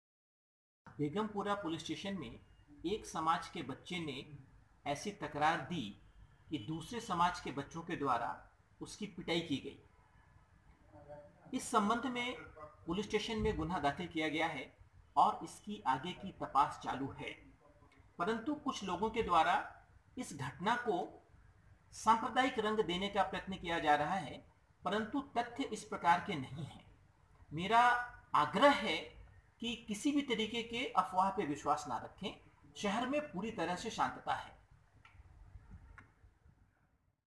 औरंगाबाद के मॉब लिंचिंग के वाक़िये पर शहर के पूलिस कमिशनर का बयान
Aurangabad City Police Commissioner IPS Chiranjeev Prasad ka city me Mob lynching ki shikayat per bayan.